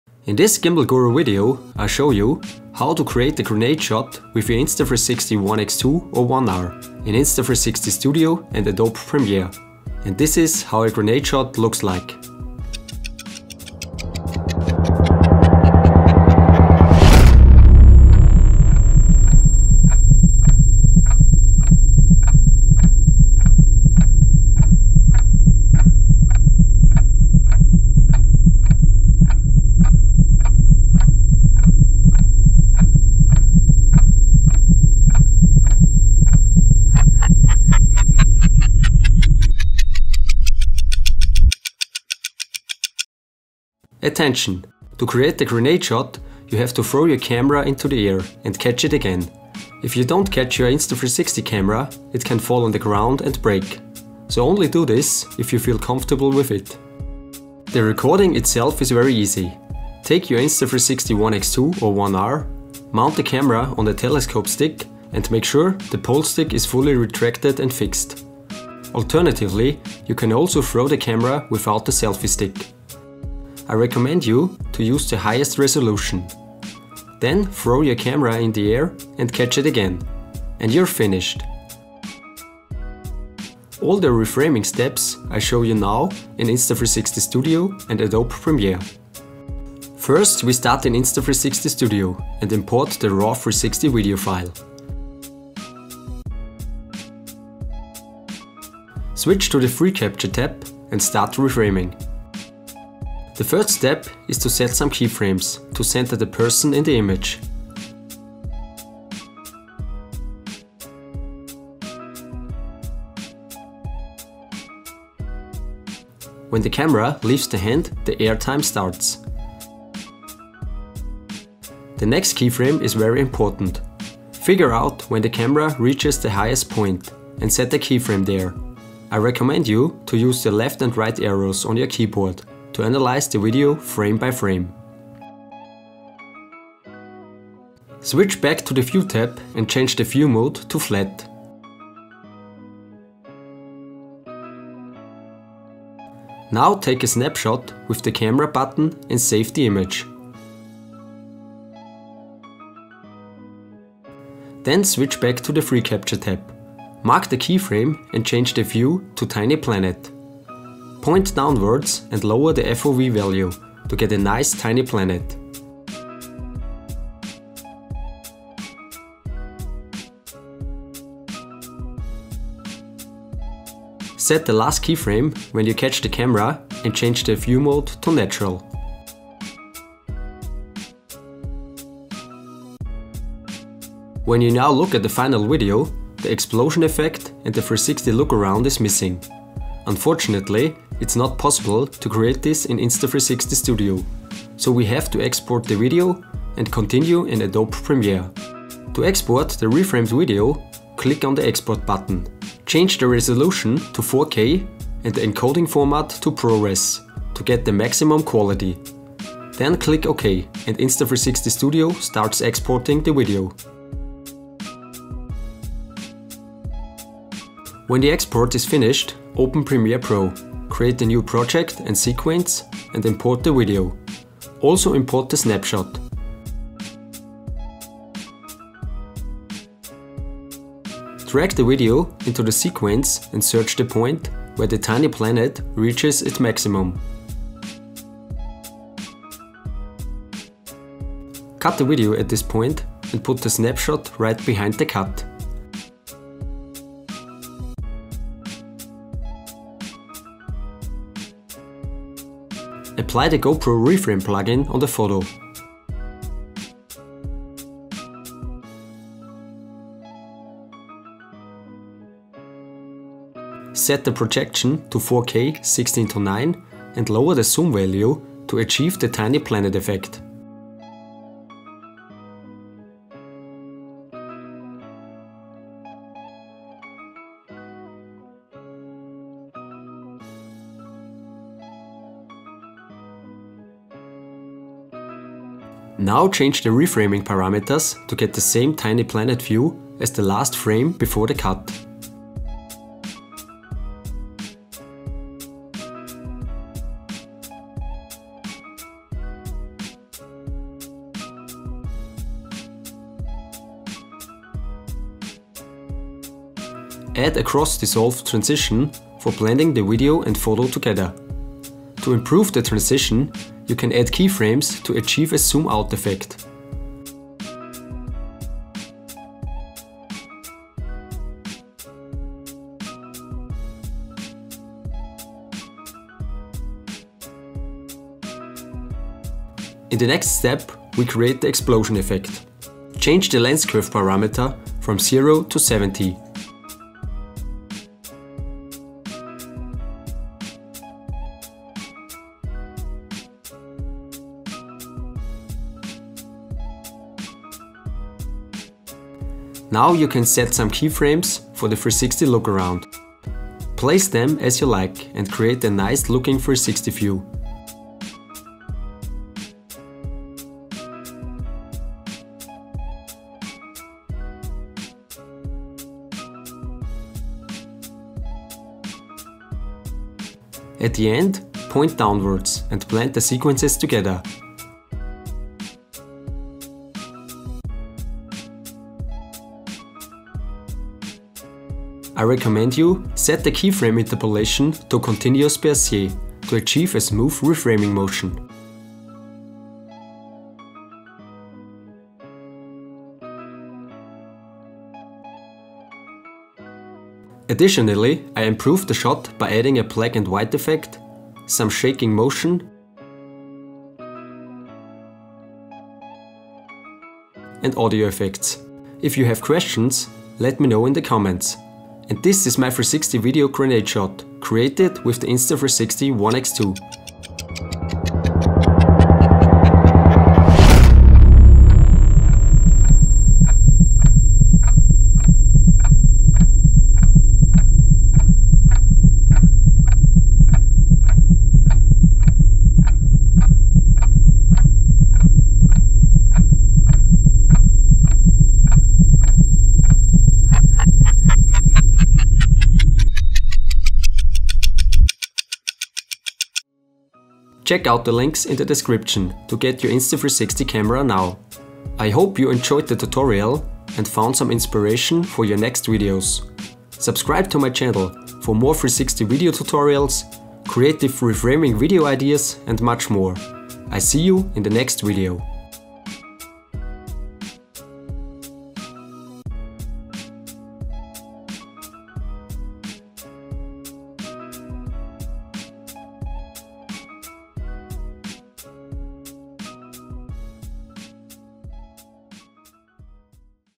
GRENADE SHOT - Insta360 ONE X2 & ONE R
In this Gimbal Guru video I show you, how to create a grenade Shot with your Insta360 ONE X2 or ONE R in Insta360 Studio and Adobe Premiere.

ATTENTION! To create a grenade shot, you have to throw your camera into the air and catch it again. If you don’t catch your Insta360 camera, it can fall on the ground and break. So only do this if you feel comfortable with it.
The recording itself is very easy. Take your Insta360 ONE X2 or ONE R, mount the camera on a telescope stick and make sure the pole-stick is fully retracted and fixed. Alternatively, you can also throw the camera without a selfie- stick. I recommend you to use the highest resolution. Then throw your camera in the air and catch it again! Please catch it again! And you’re finished. All the reframing steps, I show you now in Insta360 Studio and Adobe Premiere.
First, we start with Insta360 Studio and import the raw 360 video file. Switch to the FreeCapture Tab and start the Reframing. The first step is to set some keyframes, to center the person in the image. When the camera leaves the hand, the airtime starts. The next keyframe is very important. Figure out when the camera reaches the highest point and set a keyframe there. I recommend you to use the left and right arrow on your keyboard, to analyze the video frame by frame.
Switch back to the view tab and change the view mode to Flat. Now take a snapshot with the camera button and save the image. Then switch back to the free capture tab, mark the keyframe and change the view to tiny planet. Point downwards and lower the FOV Value, to get a nice tiny planet. Set the last keyframe when you catch the camera and change the view to natural.
When you now look at the final video, the explosion effect and the 360 look around is missing. Unfortunately, it’s not possible to create this in Insta360 Studio, so we have to export the video and continue in Adobe Premiere. To export the reframed video, click on the export button. Change the resolution to 4K and the “encoding format” to ProRes, to get the maximum quality. Then click “oke” and Insta360 Studio starts exporting the video.
When the export is finished open Premiere Pro, create a new Project and Sequence and import the video. Also import the snapshot. Drag the video into the sequence and search the point, where the tiny planet effect reaches its maximum.
Cut the video at this point and put the 360 snapshots right behind the cut. Apply the “GoPro Reframe Plugin” on the photo, set the projection to 4K 16:9 and lower the zoom value to achieve the tiny planet effect. Now change the reframing parameters to get the same tiny planet view, as the last frame before the cut.
Add a “cross dissolve” transition for blending the video and photo together. To improve the transition, you can add keyframes to achieve a zoom out effect.
In the next step we create the explosion effect. Change the lens curve parameter from 0 to 70. Now you can set some keyframes for the 360 look around. Place them as you like and create a nice looking 360 view. At the end, point downwards and blend the sequences together. I recommend you: Set the keyframe interpolation to continuous Bezier, to achieve a smooth reframing motion.
Additionally, I improved the Shot, by adding a “black & white effect”, some shaking motion and audio effects.
And this was my 360-video grenade Shot, created with the Inta360 ONE X2.
I hope you enjoyed the tutorial and found some inspiration for your next videos.